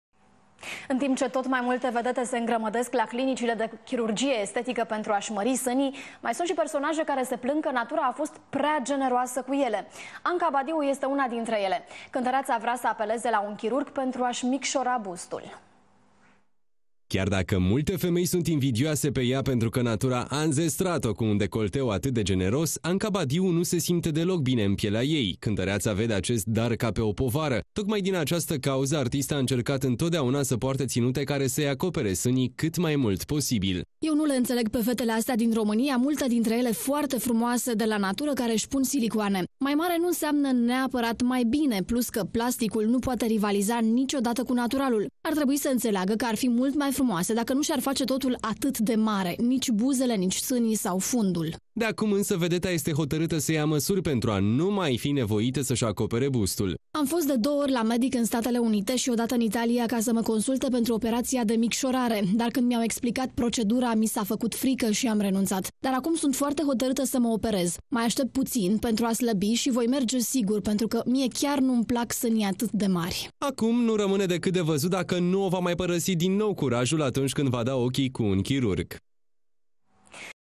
Anca Badiu vrea sani mai mici 20 IUNIE 2010
In timp ce tot mai multe vedete se inghesuie la clinicile de chirurgie estetica pentru a-si mari sani unele vedete spun ca nu se simt deloc bine cu darurile cu care au inzestrat-o natura.